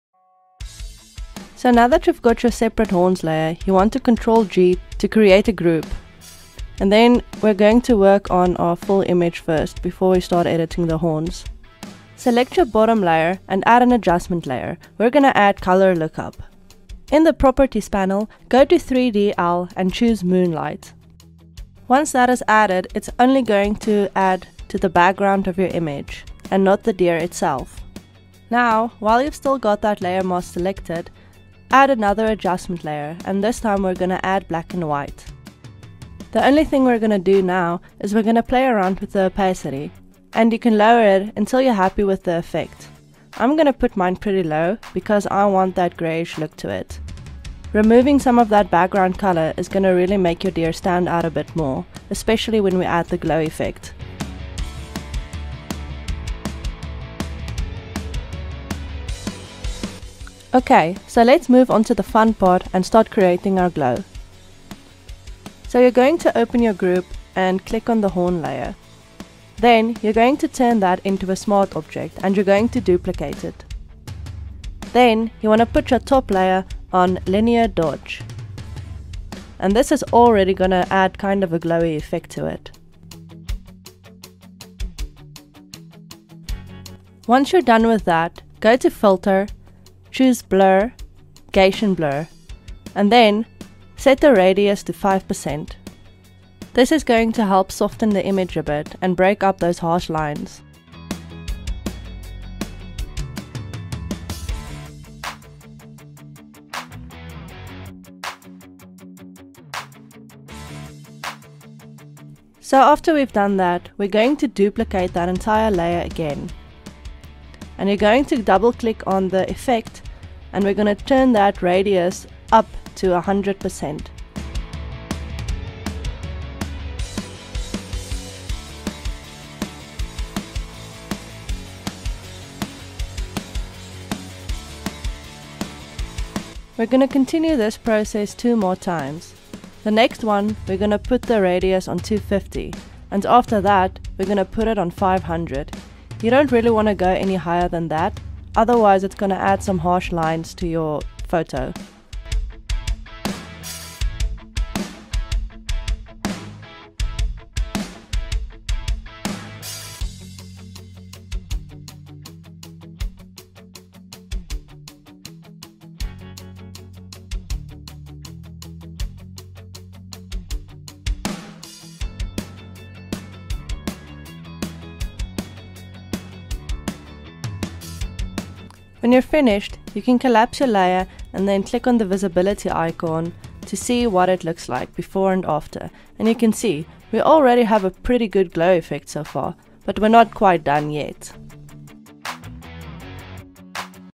How to Add a Glow Effect in Photoshop | Part3 Adding glow v1
In this Advanced Glow/Glowing Effect Photoshop course, learn how to make glowing effect in photoshop. I will teach you how to add glow to any image or object in photoshop.
i worked in Photoshop CC and use an image from Pixabay.

Cradit@
Roxane Strijdom